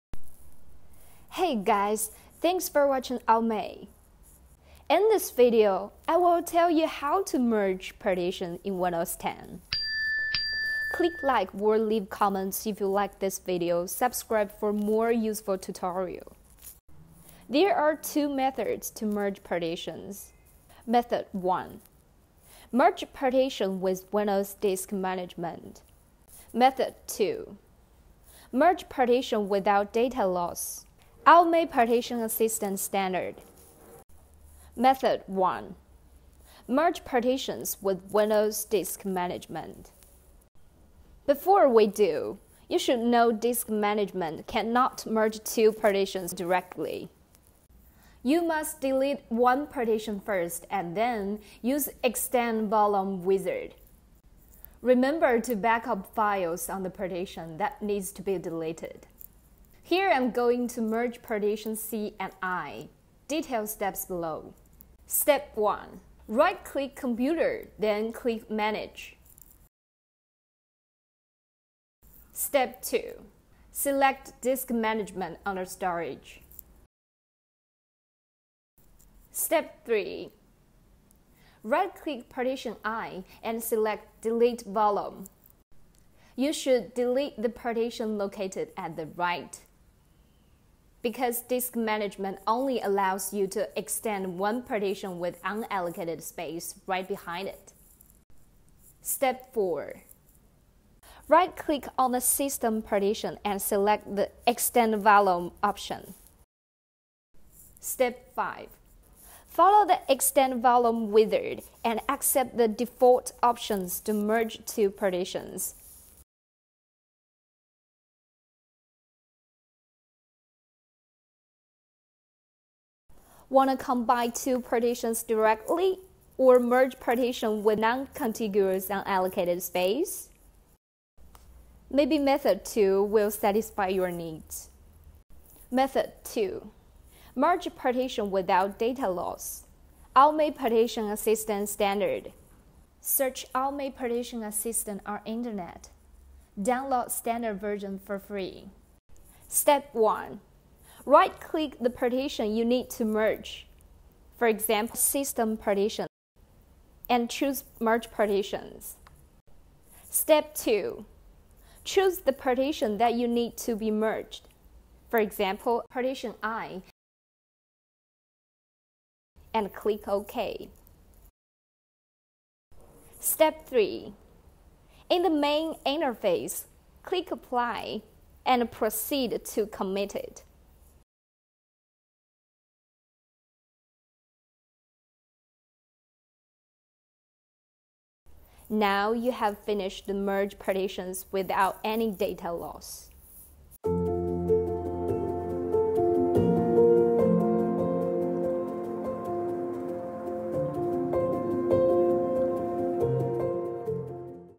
How to Merge/Combine Partitions Directly without Data Loss in Windows 10?
Merging partitions refers to combining two partitions into one partition or combining a partition with unallocated space on the hard drive to expand the space of a partition.
This tutorial provides two methods to complete merging partitions in Windows 10:
0:00 Start
0:35 Methods 1. Merge Partitions with Windows Disk Management.
2:16 Methods 2. Merge Partitions without data loss—AOMEI Partition Assistant Standard.